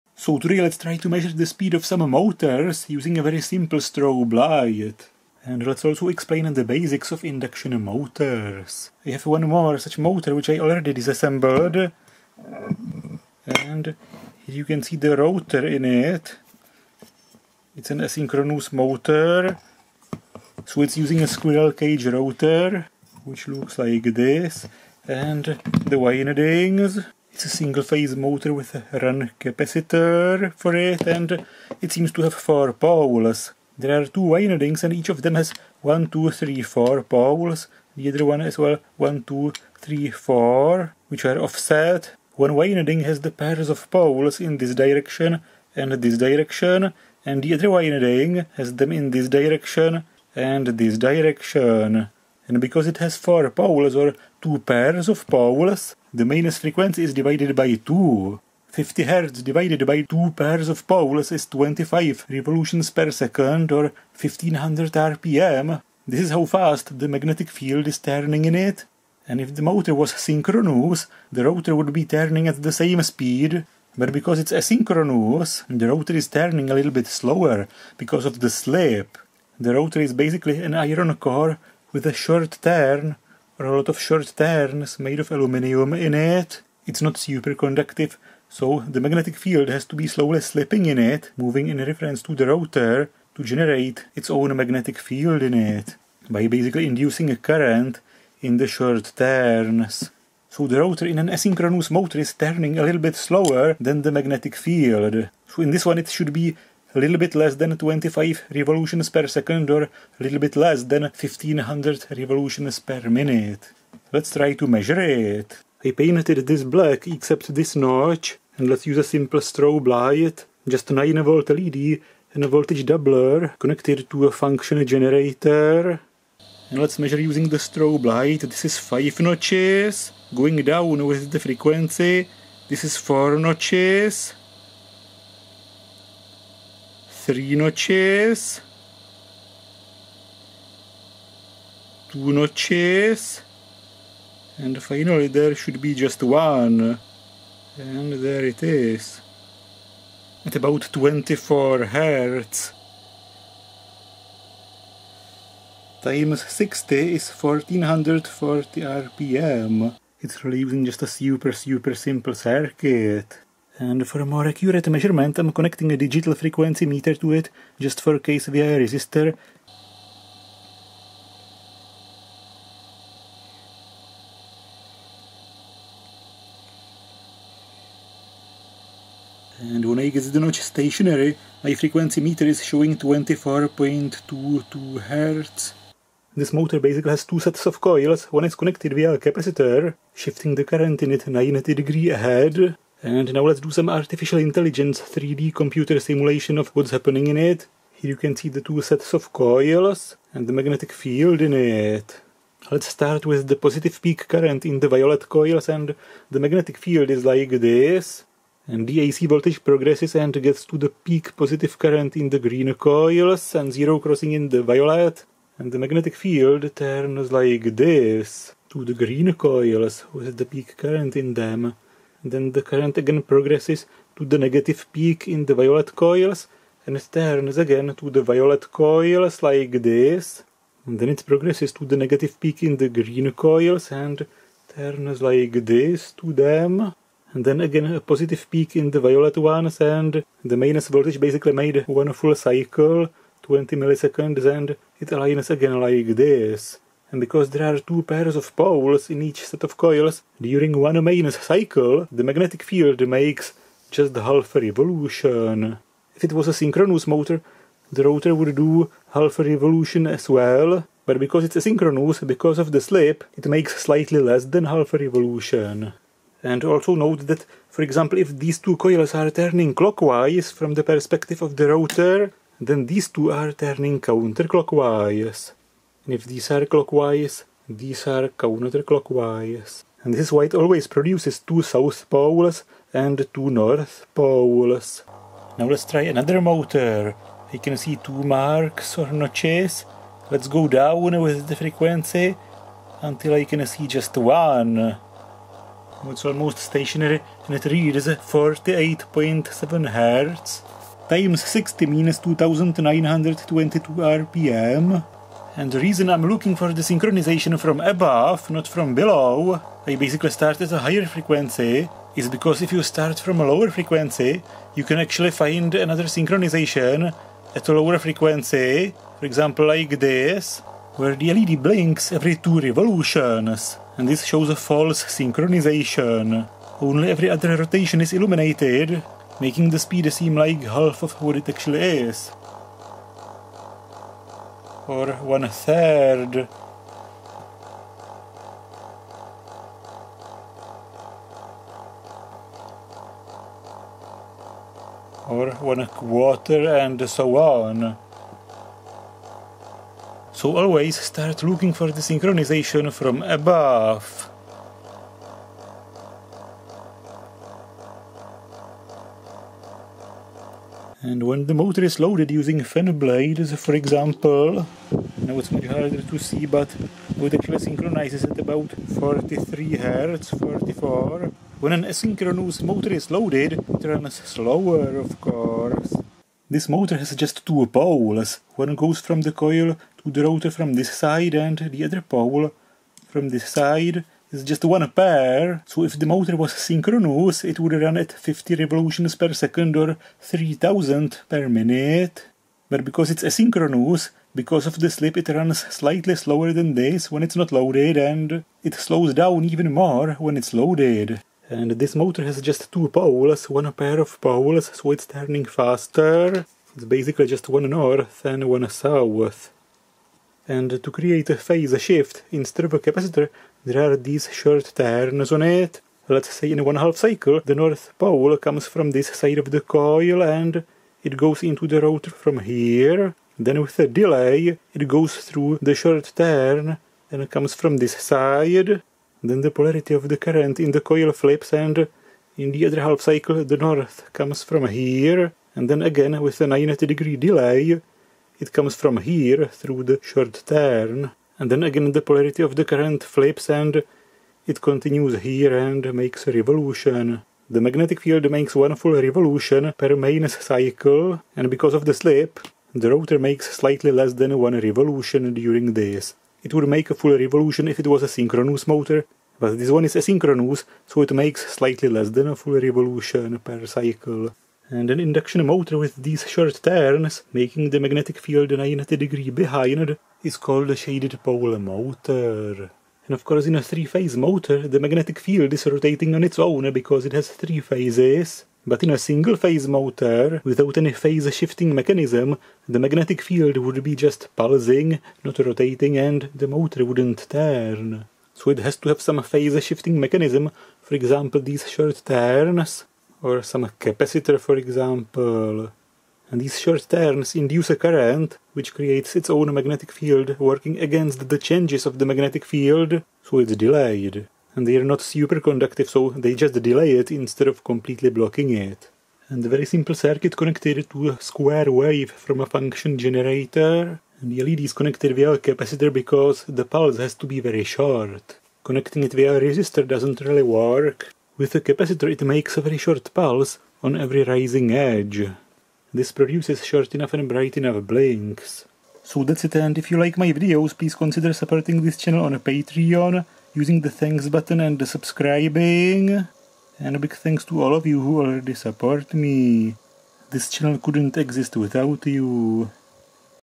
Induction motors + strobe light RPM measurement
Explaining how an induction AC motor works (synchronous / asynchronous). Measuring the speed of some asynchronous motors using a strobe light method. Connecting and LED to a function generator and a frequency meter to create an accurate RPM meter.